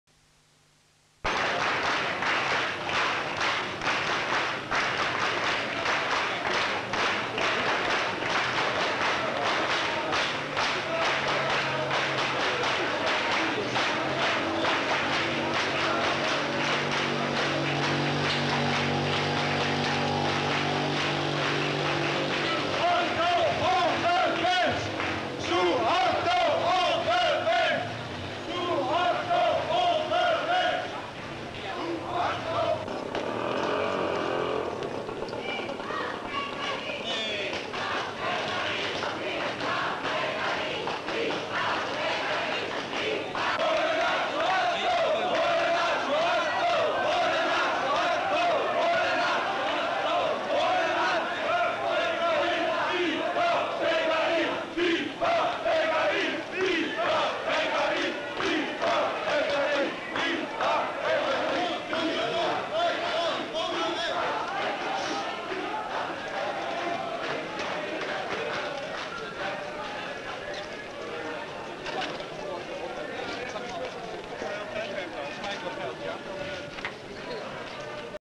News Item (4b77e671-838a-45ba-f352-69f85daca859)
(1 Sep 1970) 09/02/70   c0013033 / color
 amsterdam:  dutch communists and pacifists mound a demonstration in amsterdam protesting against the impending visit of president suharto of indonesia:

 lnc 16044 "anti - visit march" shows:  ms demonstrators marching through amsterdam with banners, shouting slogans and clapping hands in unison:

 anti - indonesia - netherlands
 indonesia - for rel - netherlands
 netherlands - for rel - indonesia
 suharto, pres - visit opposed

 upitn / 49 ft / 16 col / pos / r30875